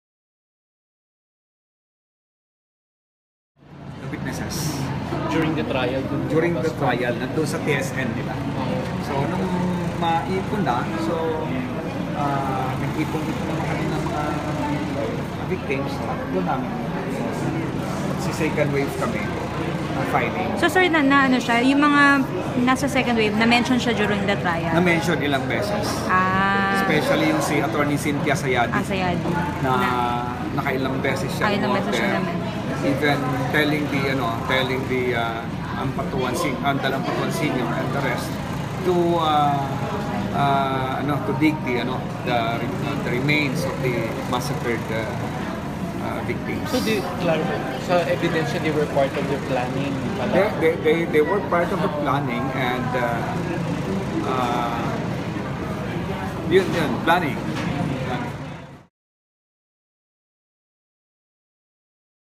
14 more Ampatuans including mayor of Mamasapano tagged in 2009 massacre
Gov. Toto Mangudadatu said the new batch of respondents would not affect the 58 counts of murder already pending before the Quezon City Regional Trial Court Branch 221.